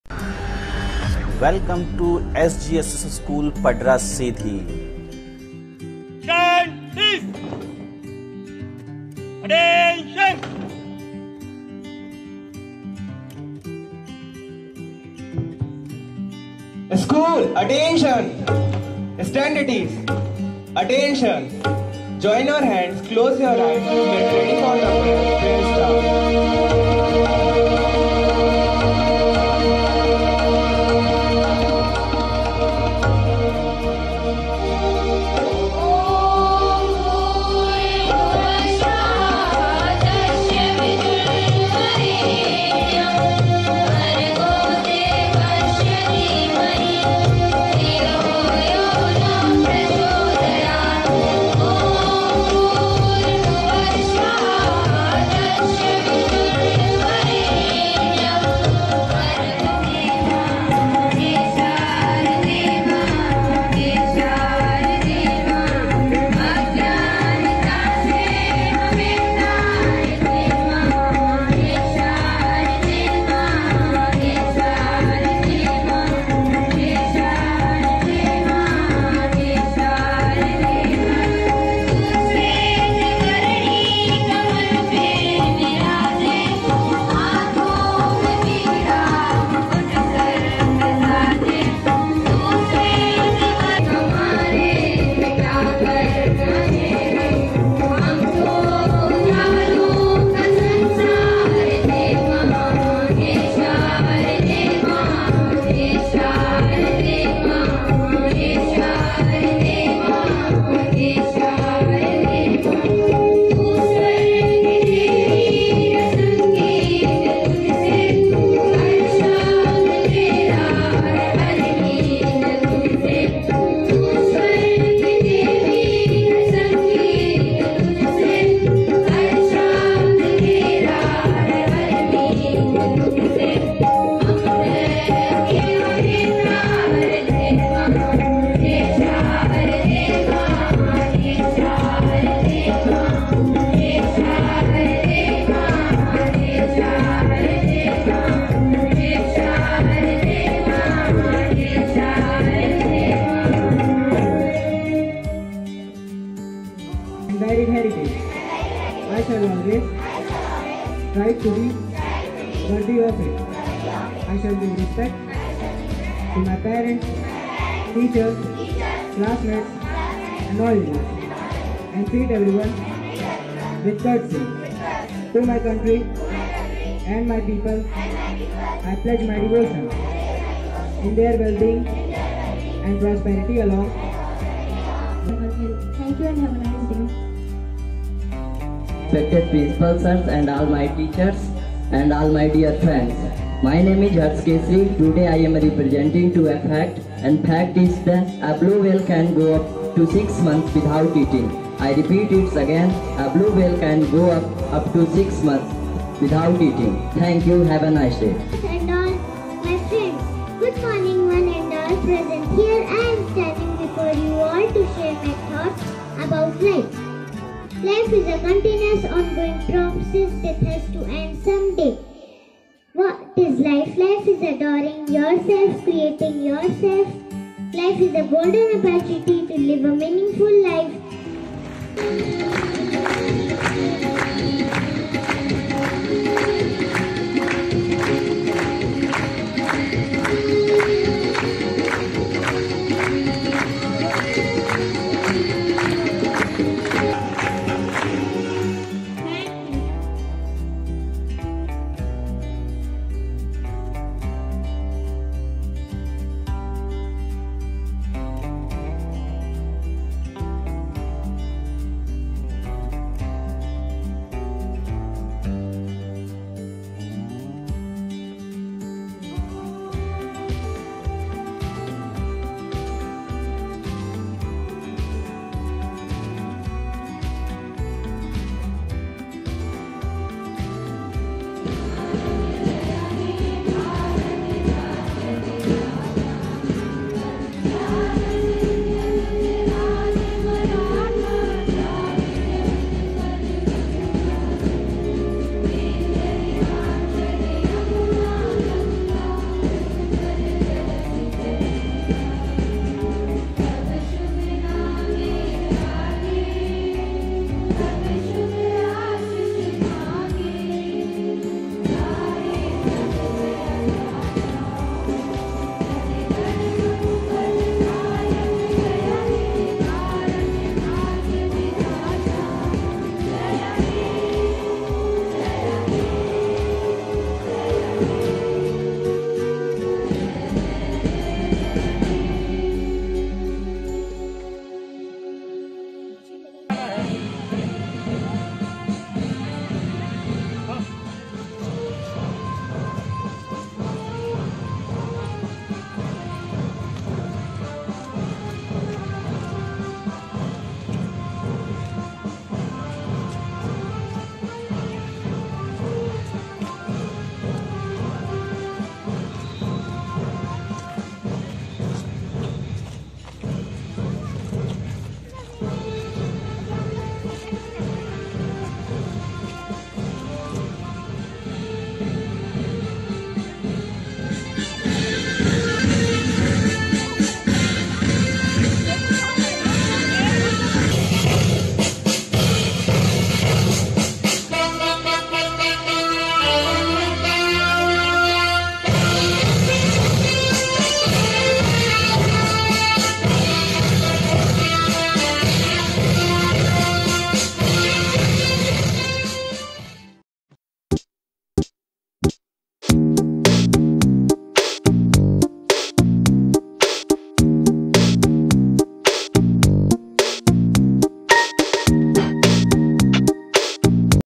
Morning Assembly | Shri Ganesh Sr. Sec. School | Sidhi (M.P.)
Morning Assembly | Shri Ganesh Sr. Sec. School

Morning Assembly
Morning Assembly is the time when the entire school community assembles at an appropriate place to affirm school’s identity and aspiratons. It is the best forum to inspire and motivate students. It serves as a platform to develop self confidence, acquiring knowledge and information, creativity and a esthetic sense among students.

Morning is the perfect time to revive the learning, take in inspiration and begin your day with positivity. As the students walk into the school campus the first activity that kick starts the day are the Special Morning Assembly.

In the serene hours of the morning, the students are made to assemble on the ground and pray to the Lord for their guidance and wisdom. The prayer is followed by guide from mentors, skits and displays of slogans, important notices, speech, songs, National Anthem and even the plan for the day.